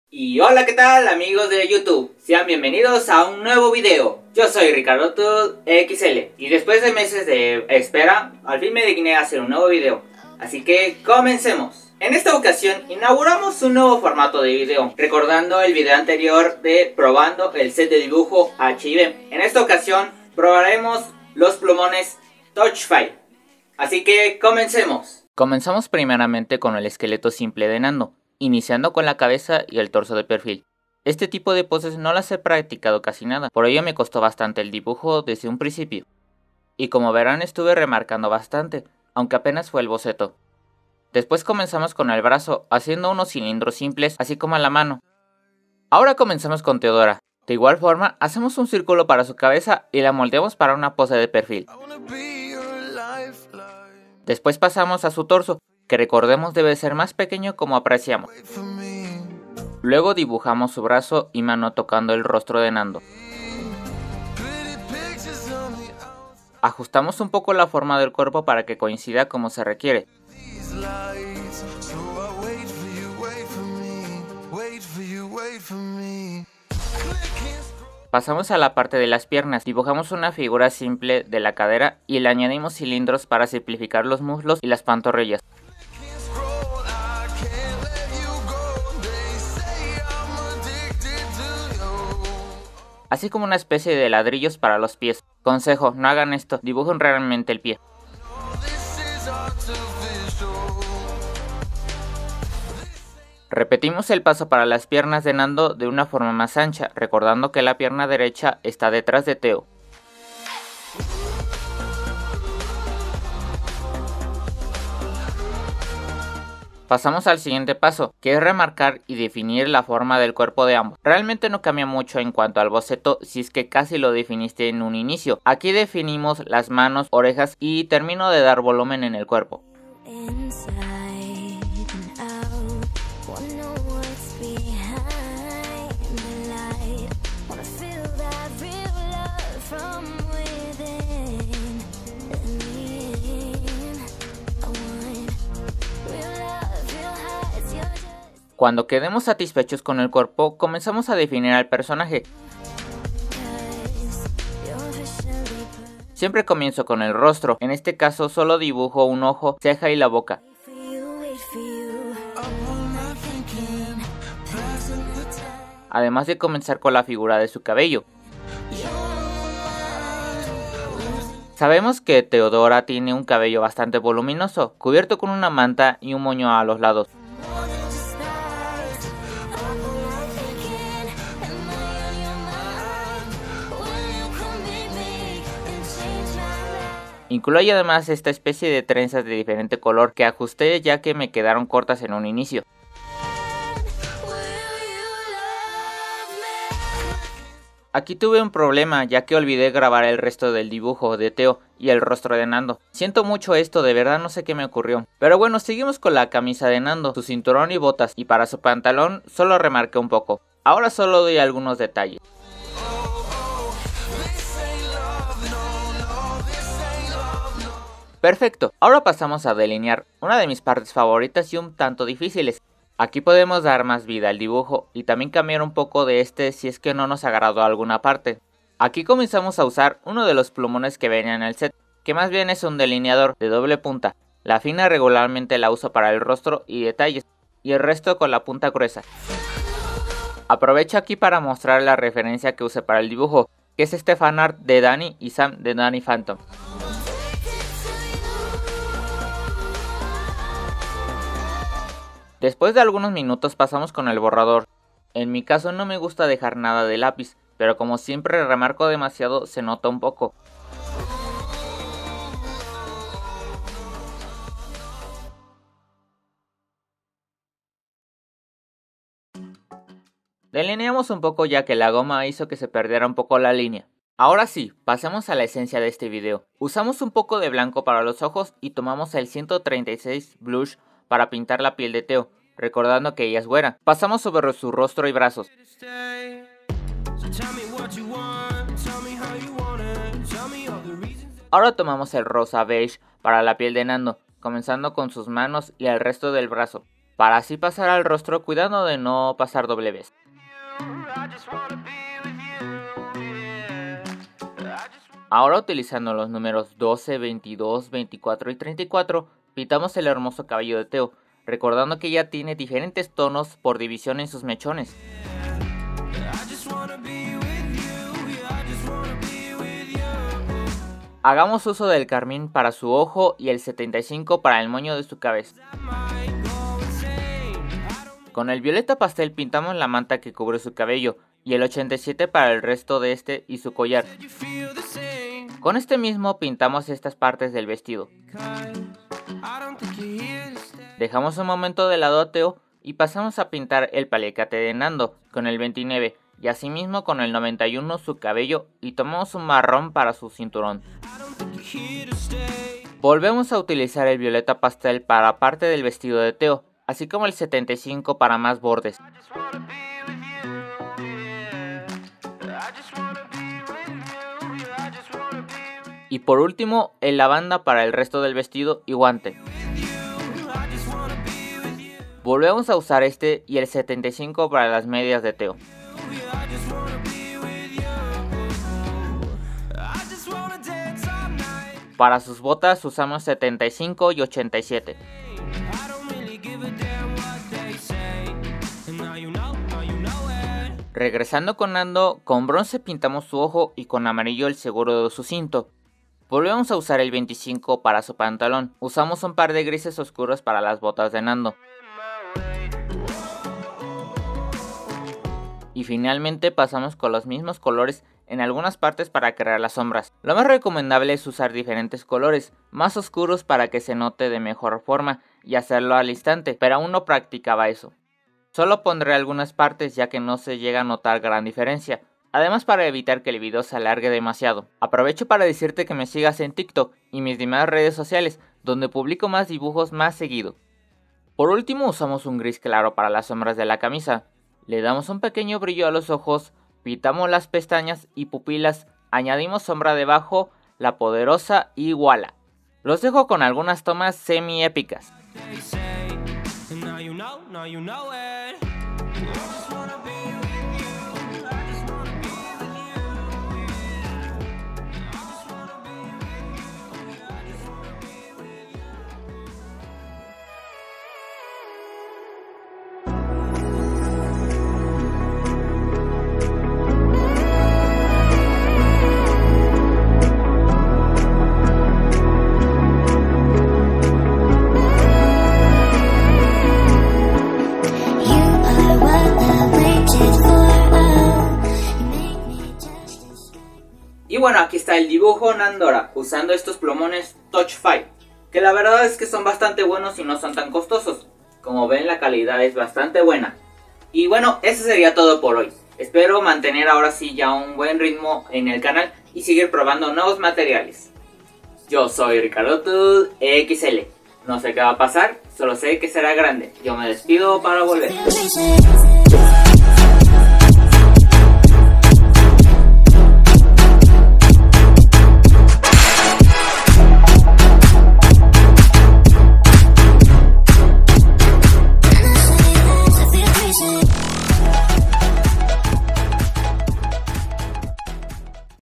Dibujo NANDORA- Narrado | Probando marcadores Touch Five
¿Qué tal amigos? Hoy les traigo un nuevo vídeo. En esta ocasión realizamos un dibujo Nandora, donde además probaremos los marcadores Touch Five.